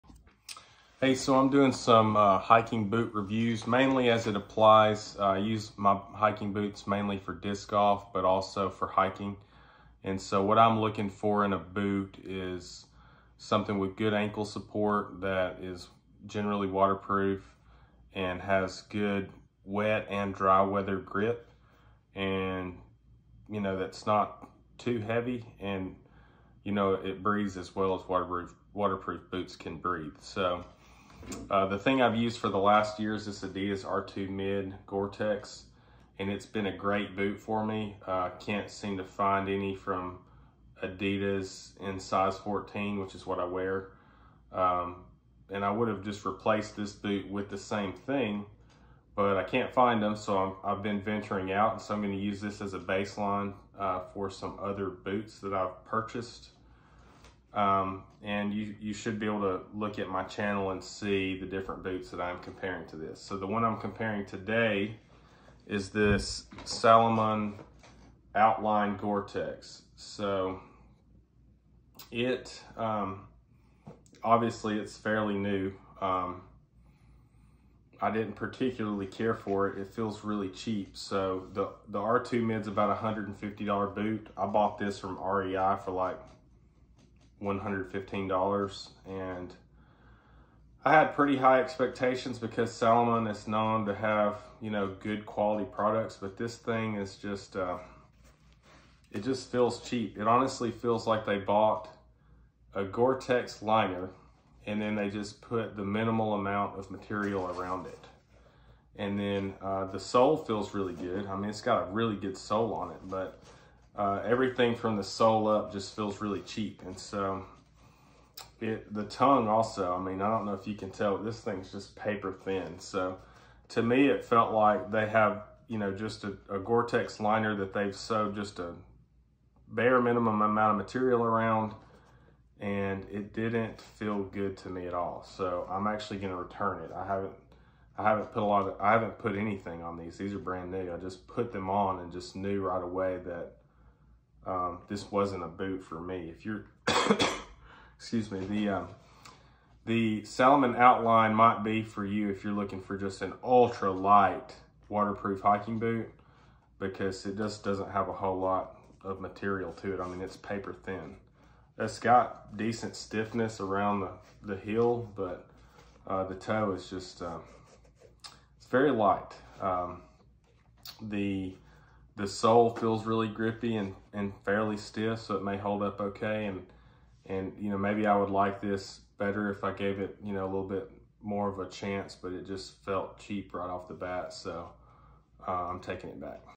Adidas R2 mid vs Salomon Outline GTX
In this video, I compare the excellent Adidas R2 mid goretex (GTX) to the Salomon Outline GTX.